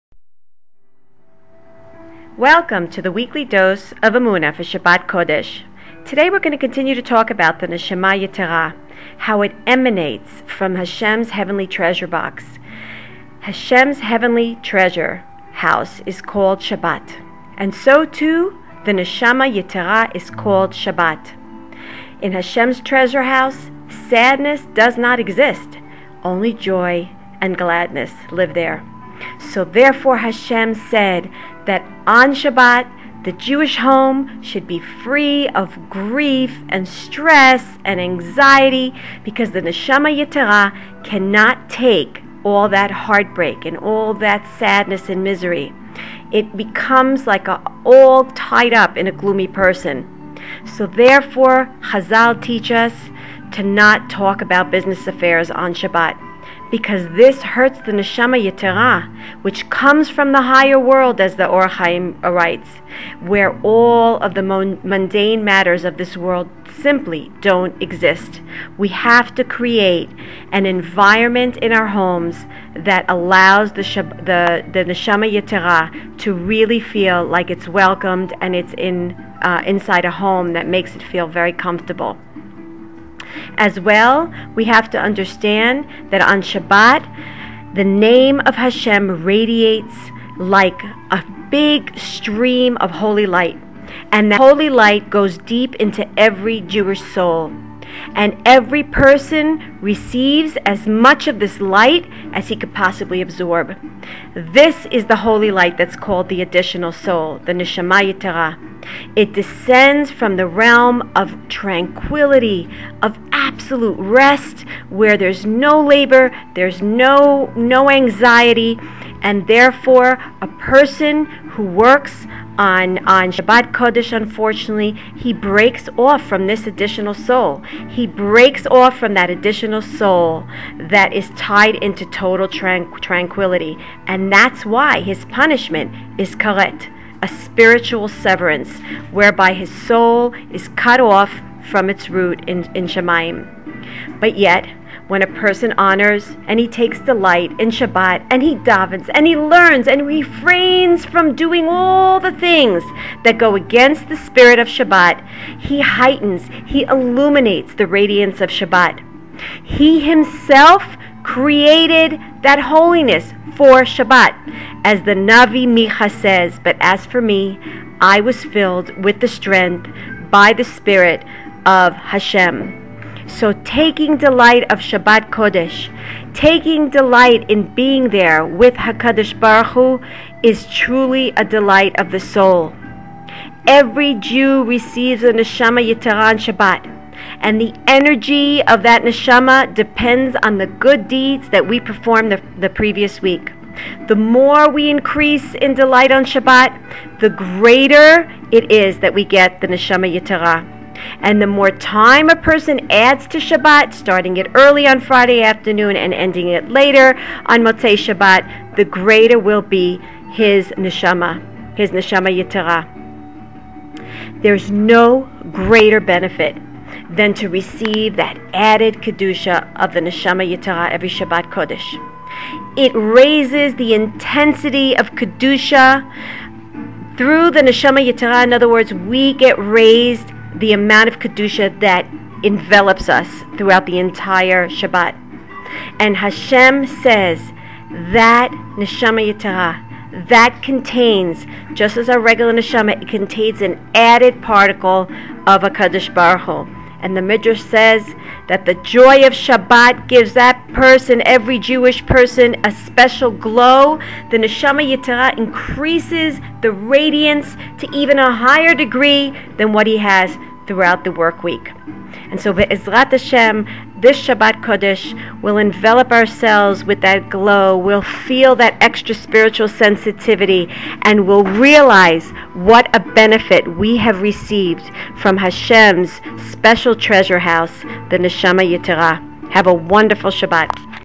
Shabbat Audio The Gift of The Additional Soul Part 2 By Orit Riter
Strictly for women only.
This Lecture was recorded at Jun. 3, 2015
Ramat Beit Shemesh, Israel